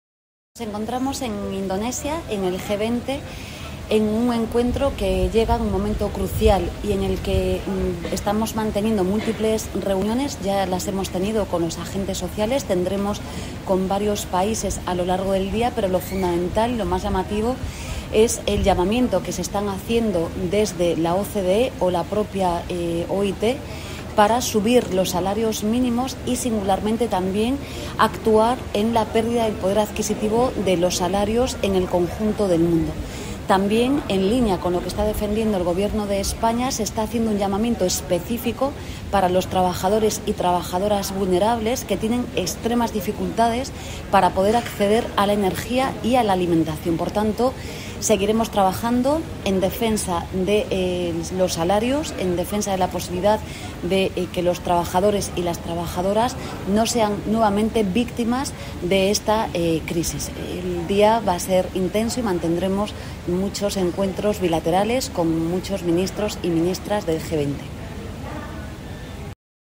Declaraciones de la Ministra de Trabajo, Yolanda Díaz, en el G20
Declaraciones de la Ministra de Trabajo en el G20 celebrado en la India. Yolanda Díaz defiende en el G20 la subida de salarios, en línea con lo expresado por la OCDE y la OIT.